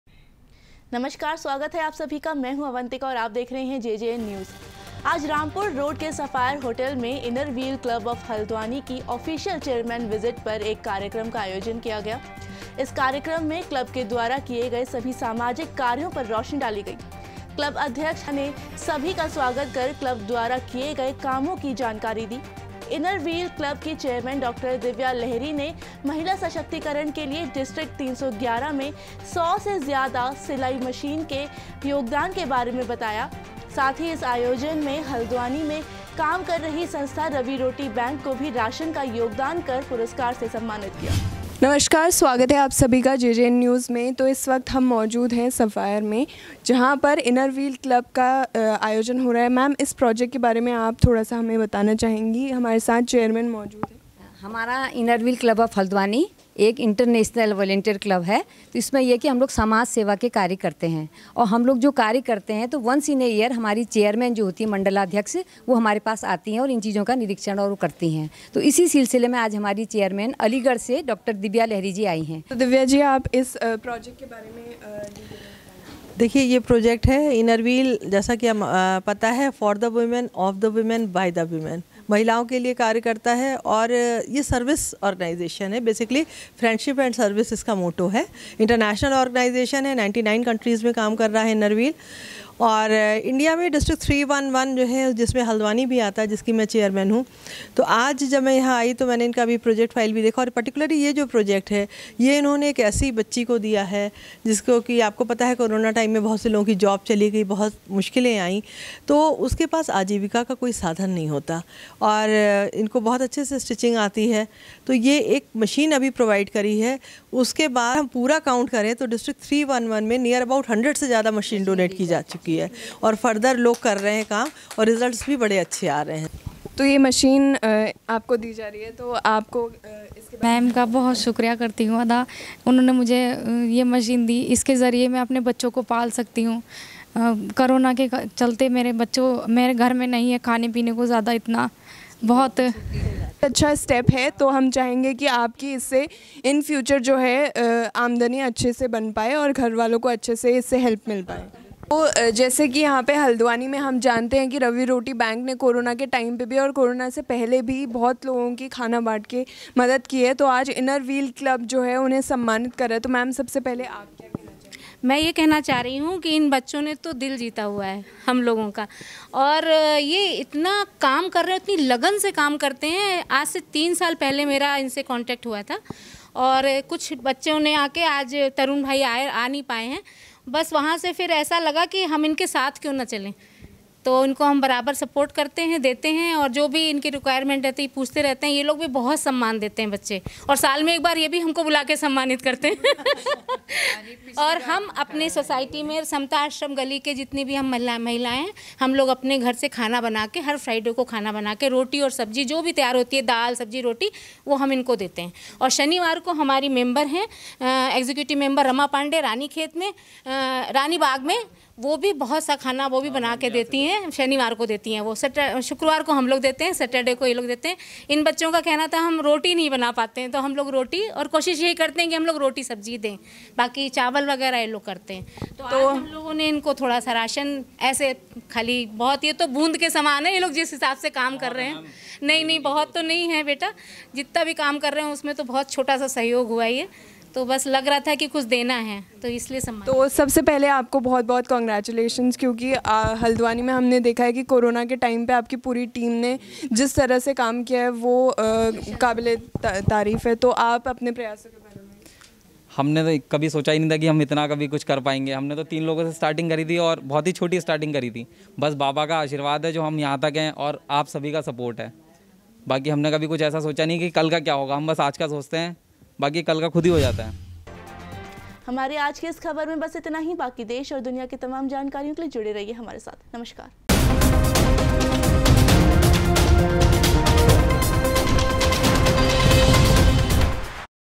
हल्द्वानी के इनर व्हील क्लब और राइजिंग स्टार्स ने आयोजित किया चेयरमैन के लिए ये कार्यक्रम|| JJN News
JJN News Nainital,
JJN News,